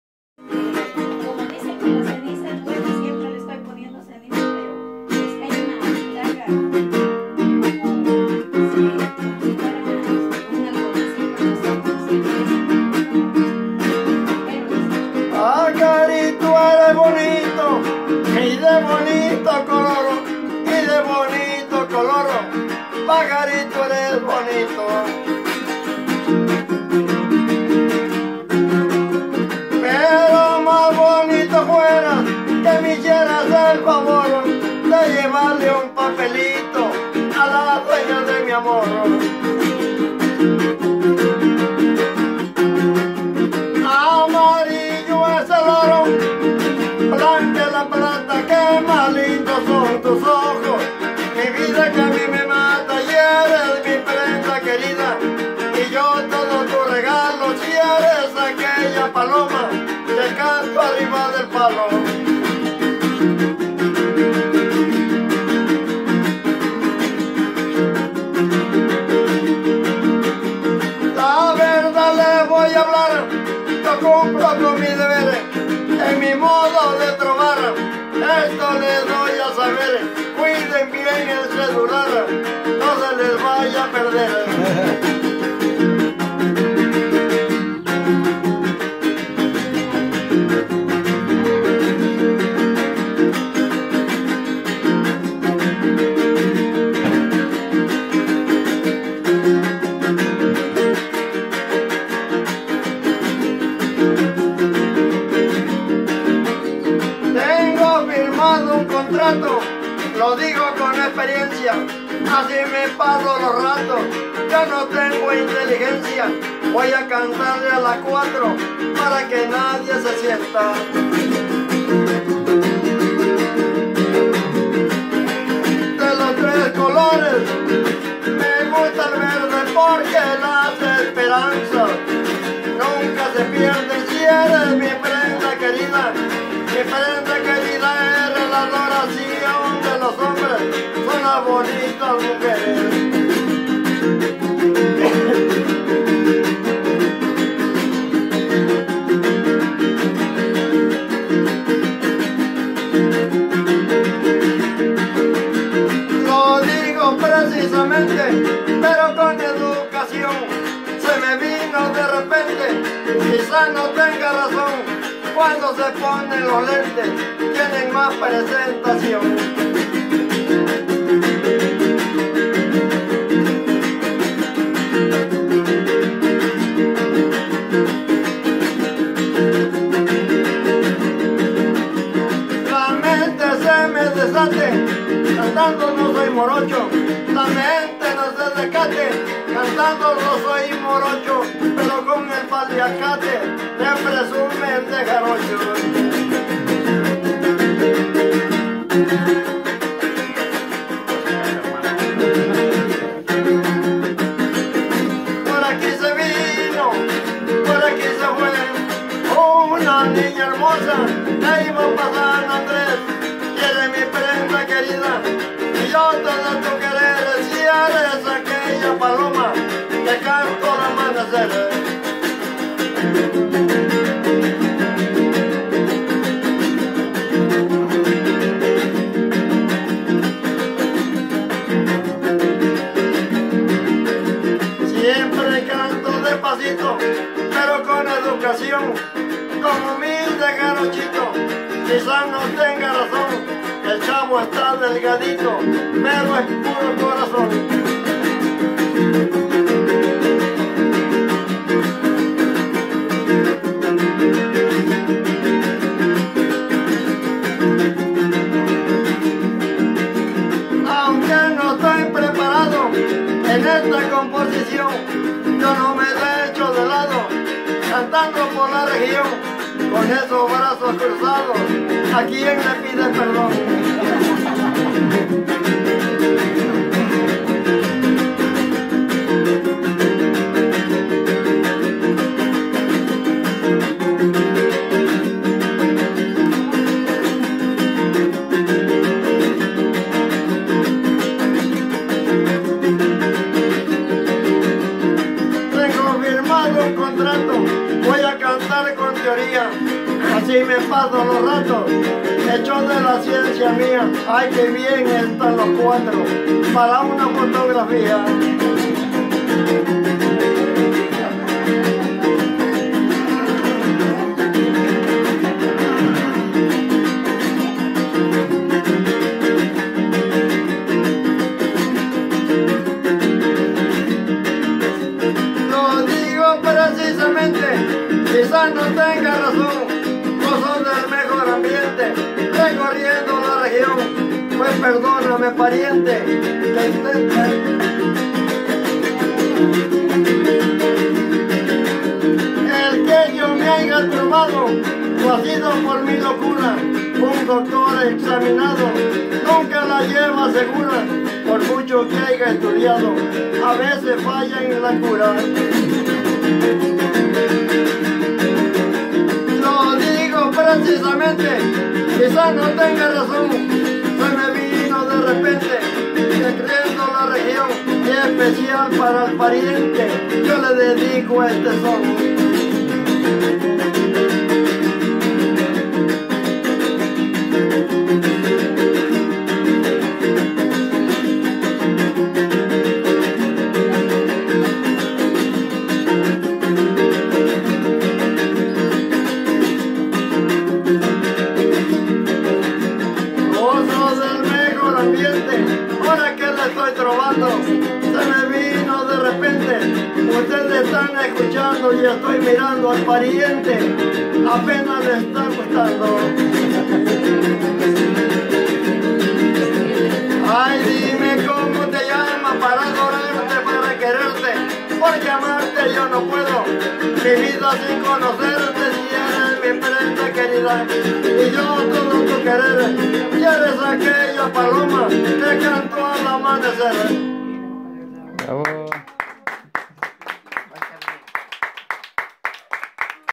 El Chato jaranero. El Chicle Colonia del Valle, Veracruz
Don Filiberto mejor conocido por "EL CHATO" Municipio Felipe Carillo Puerto, Comunidad el Colonia del Valle: EL CHICLE 2018.
JARANERO Y Músico de la región, trovador, versador, decimista.